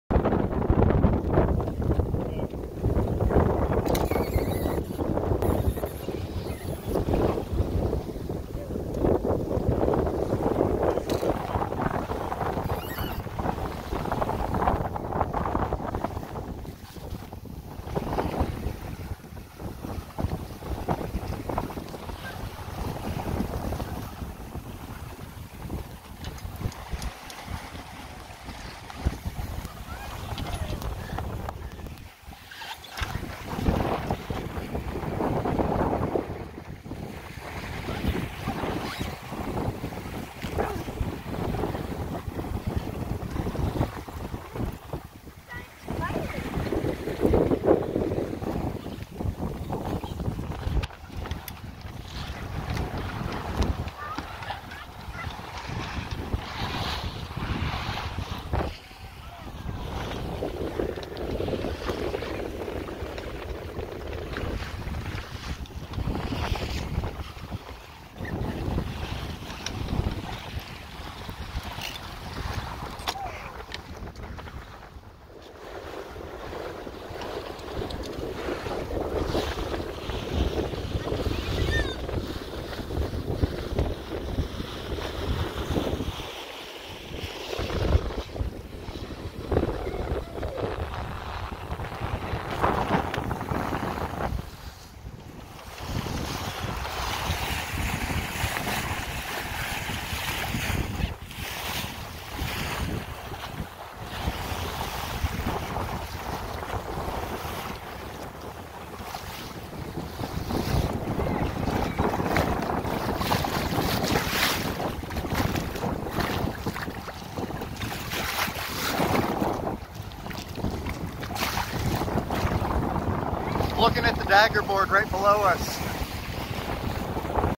Macgregor 26D... testing newly built rudder on Presque Isle Bay
1987 26D, Three Hour Tour out for a quick rip around the bay.   10 to 15 mph wind.